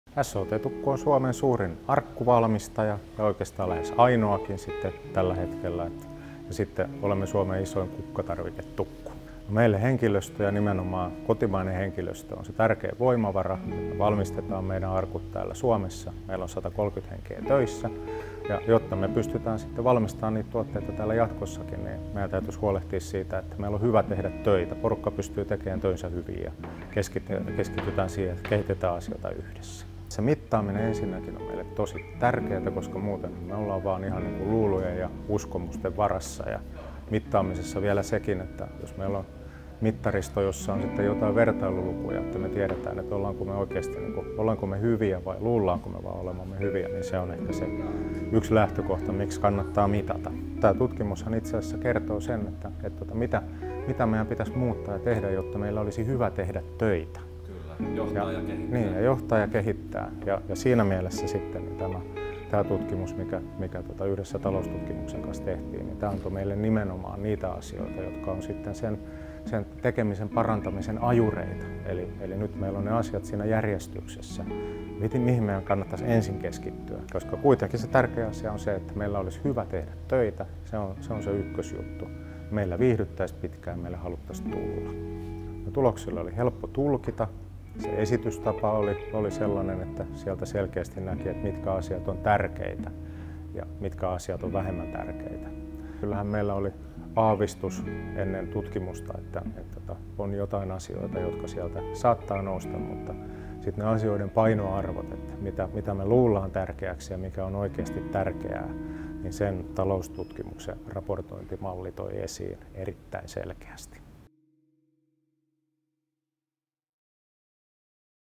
Henkilöstökokemus – Uusi tapaa tietää⎪SHT-Tukku⎪Duunitori ja Taloustutkimus
"Tärkeintä on se, että meillä olisi hyvä tehdä töitä", toteaa SHT-Tukun toimitusjohtaja Pekka Kivimaa. Katso videolta, miten Taloustutkimuksen kanssa tuotettu Henkilöstökokemus-tietopalvelu auttaa SHT-Tukkua henkilöstökokemuksen mittaamisessa ja kehittämisessä.